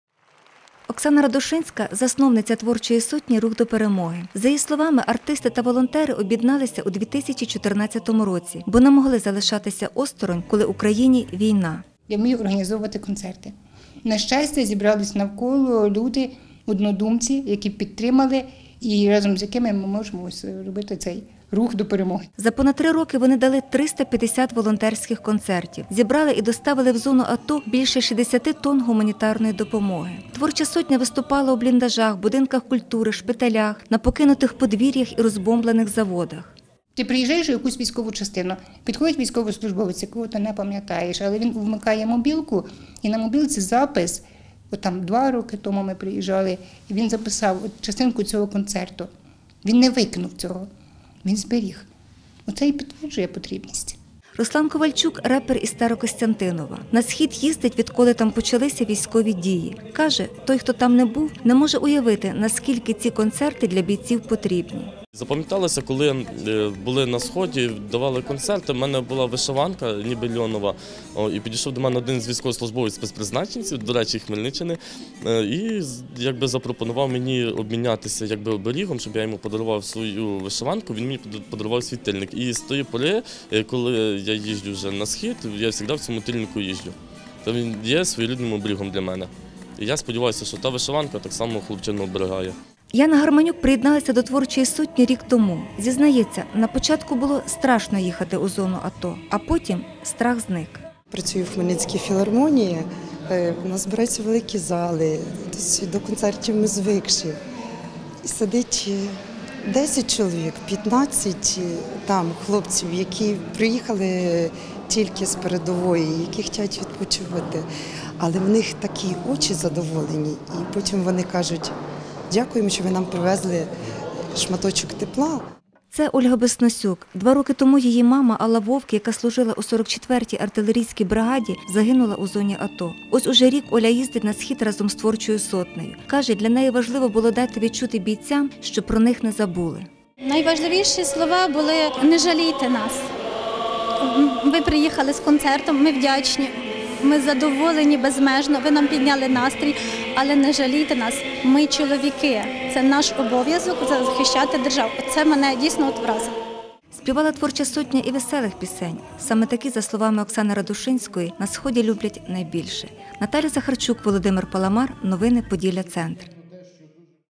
сюж Захарчук Творча сотня «Рух до перемоги» виступила у Хмельницькому
Творча сотня «Рух до перемоги» виступила у Хмельницькому
Волонтери творчої сотні «Рух до перемоги»виступили у Хмельницькому. Концерт під назвою «Ми переможемо» назвали своєрідним творчим звітом перед земляками.  За словами засновниці творчої сотні Оксани Радушинської, артисти хотіли подякувати усім, хто їх підтримує. Впродовж останніх 3 років волонтери дали 350 концертів у зоні АТО, відвезли бійцям на Схід 60 тонн гуманітарної допомоги. А от у Хмельницькому творча сотня виступила вперше. Більше розкаже Наталя Захарчук.